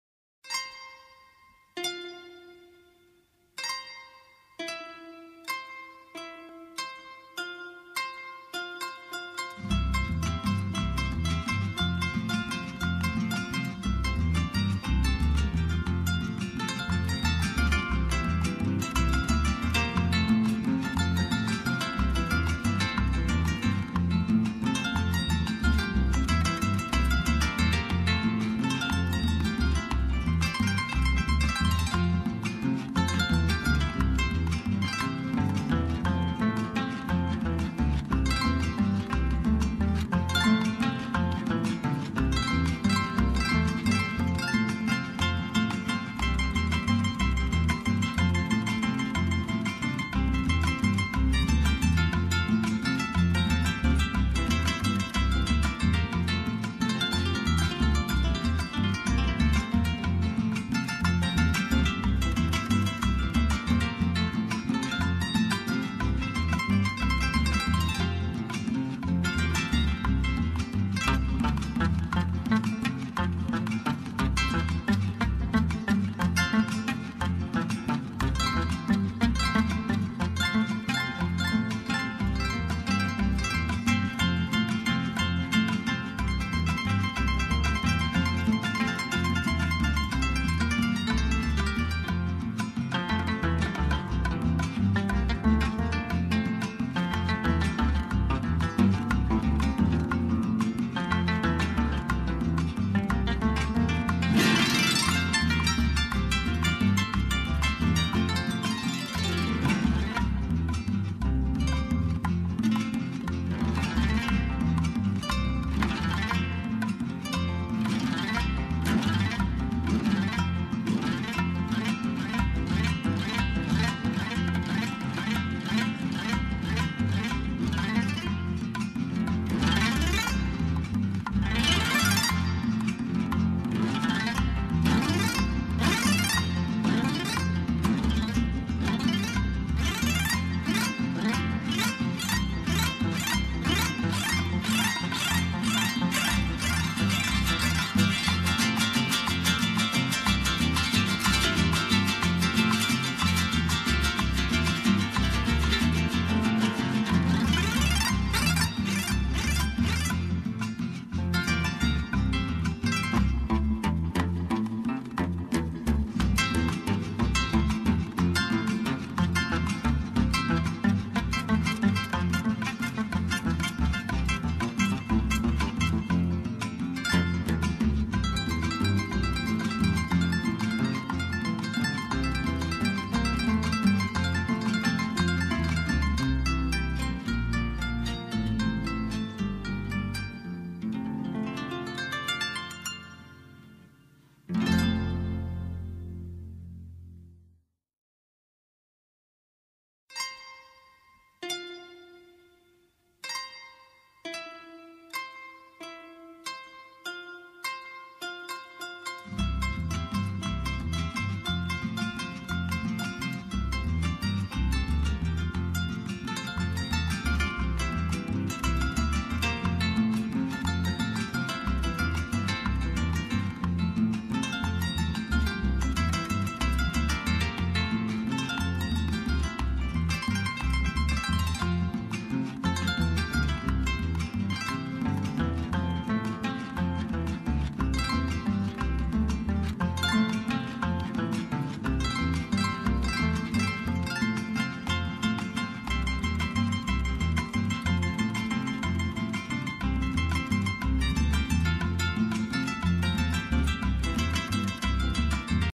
El CAMPO - Fields
a walk outside on a windy day - enjoy